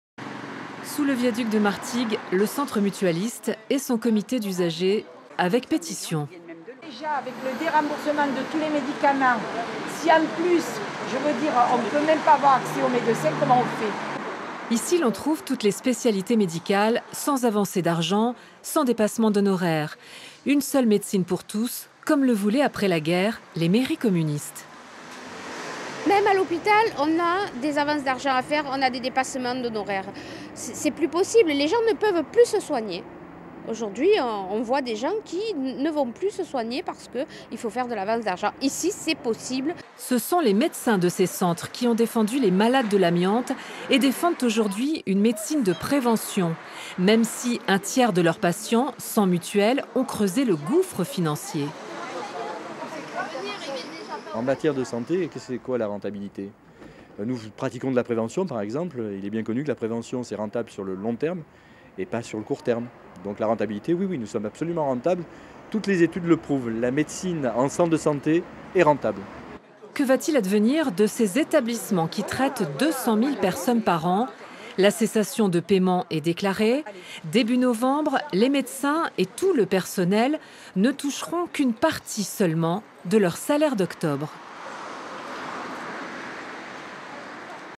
Pétion pour sauver les centres mutualistes
En cessation de paiement, les onze centres mutualistes des Bouches-du-Rhône se mobilisent avec leur patients. C'est le cas au centre de Martigues